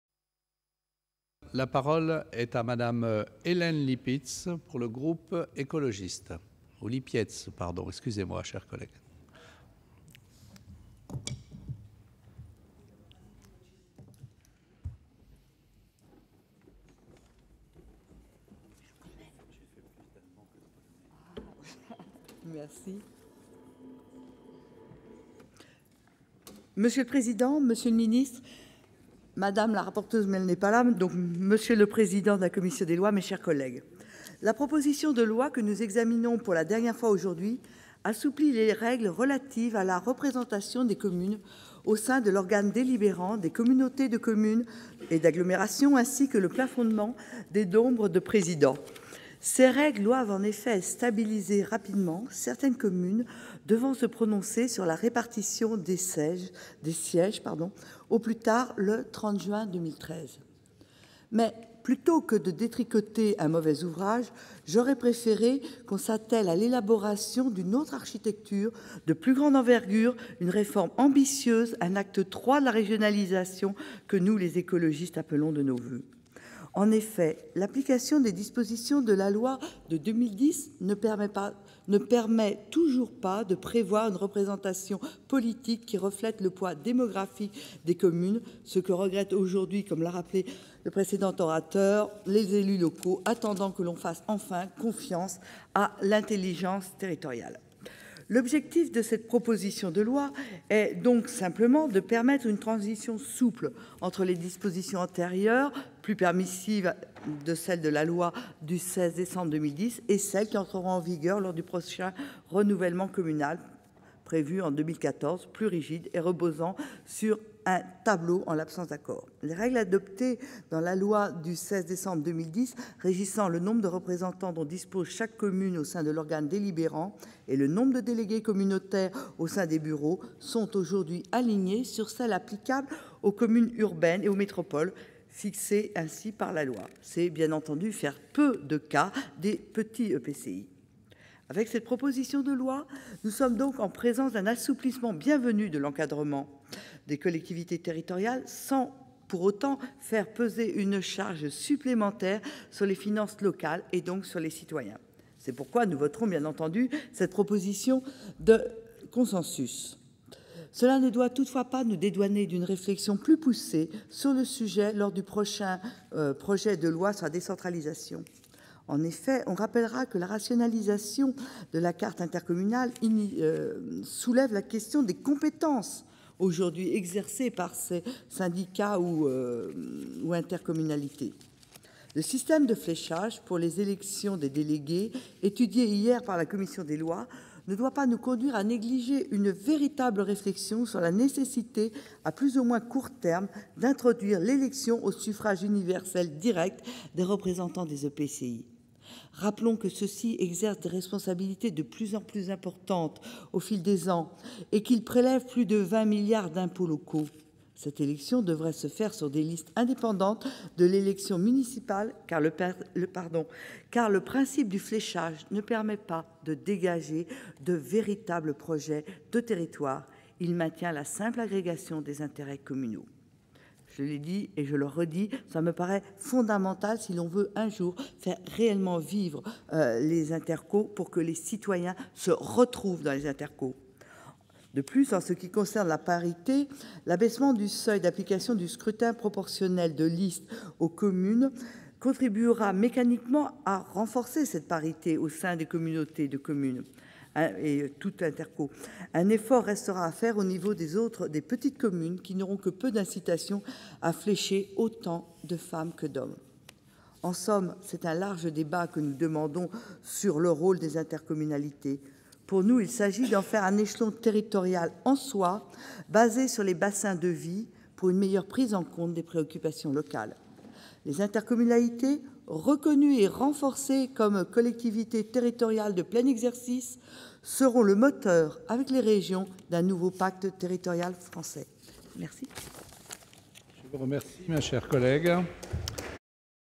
Hélène Lipietz Représentation des communes dans les intercommunalités 2ème lecture
L'intervention d'Hélène Lipietz pour la 2ème lecture au Sénat sur le texte définissant la représentation des communes dans les intercommunalités.